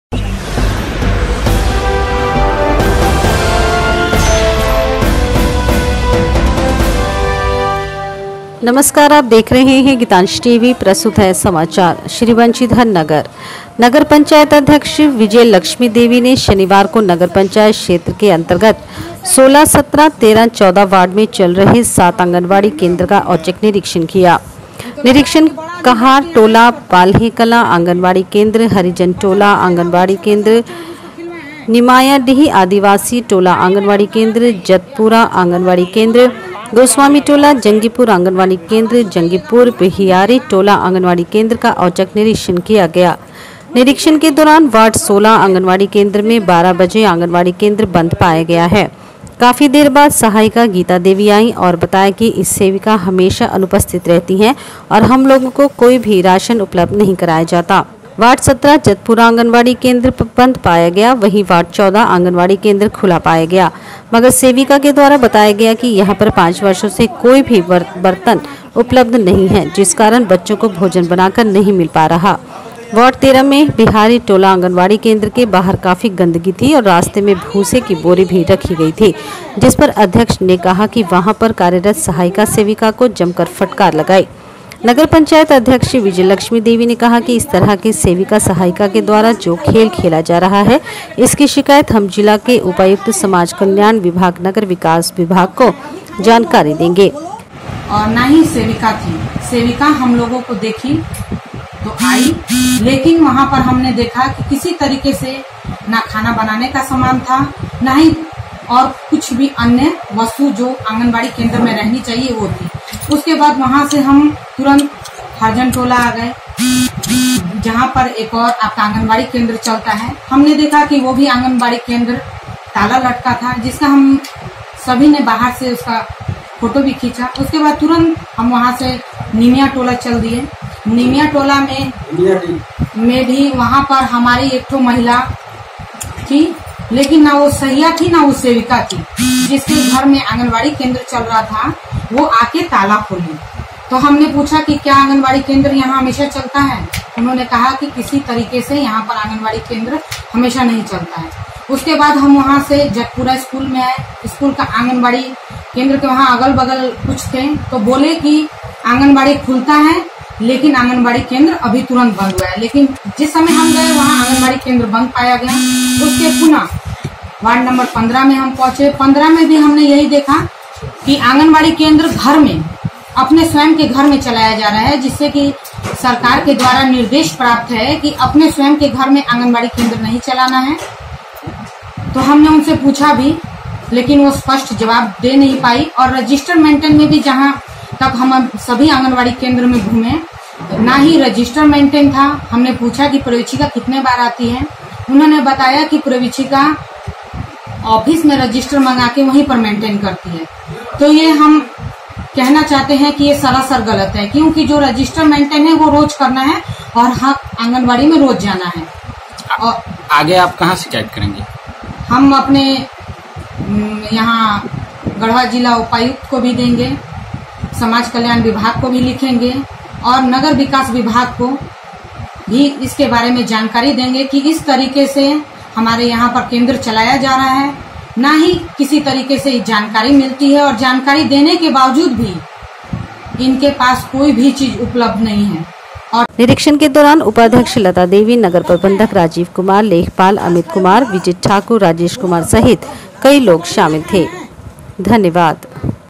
नगर पंचायत अध्यक्ष विजयालक्ष्मी देवी ने सात आंगनबाड़ी केंद्र का औचक निरीक्षण किया।
श्री बंशीधर नगर नगर पंचायत अध्यक्ष विजयालक्ष्मी देवी ने शनिवार को नगर पंचायत क्षेत्र के अंतर्गत 16, 17,  13 ,14 वार्ड में चल रहे सात आंगनबाड़ी केंद्र का औचक निरीक्षण किया। निरक्षण कहार टोला पालहे कलां  आंगनबाड़ी केंद्र, हरिजन टोला  आंगनबाड़ी केंद्र, निमायाडीह आदिवासी टोला आंगनबाड़ी केंद्र, जतपुरा आंगनबाड़ी केंद्र , गोस्वामी टोला जंगीपुर आंगनवाड़ी केंद्र जंगीपुर , बहियारी टोला आंगनबाड़ी केंद्र  का औचक  निरीक्षण किया गया ।निरीक्षण के दौरान  वार्ड 16 आंगनबाड़ी केंद्र में 12:00 बजे आंगनवाड़ी केंद्र बंद पाया गया है काफी देर बाद  सहायिका गीता देवी आए और बताया कि इस सेविका हमेशा अनुपस्थित रहती हैं और हम लोग को कोई भी राशन उपलब्ध नहीं कराया जाता है  वार्ड 17 जतपुरा आंगनबाड़ी केंद्र बंद पाया गया वहीं वार्ड 14 आगनबाडी केंद्र खुला पाया गया मगर सेविका के द्वारा बताया गया कि यहां पर 5 वर्षों से कोई भी बर्तन उपलब्ध नहीं है जिस कारण बच्चों को भोजन बनाकर नहीं मिल पा रहा है।  वार्ड 13 में है बिहारी टोला  आगनबाडी केंद्र  के बाहर काफी गंदगी  था और  रास्ते में भूसा का बोरी भी रखा गया था जिस पर अध्यक्ष ने हैं  वहां पर  कार्यरत सहायिका सेविका को जमकर फटकार लगाई ।नगर पंचायत अध्यक्ष विजयालक्ष्मी देवी ने कहा कि इस तरह के सेविका सहायिका के द्वारा जो खेल खेला जा रहा है इसकी शिकायत हम जिला के उपायुक्त समाज कल्याण विभाग नगर विकास विभाग को जानकारी जानकारी देंगे। निरक्षण के दौरान उपाध्यक्ष लता देवी नगर प्रबंधक राजीव कुमार लेखापाल अमित कुमार विजय ठाकुर राजेश कुमार सहित कई लोग  शामिल थे।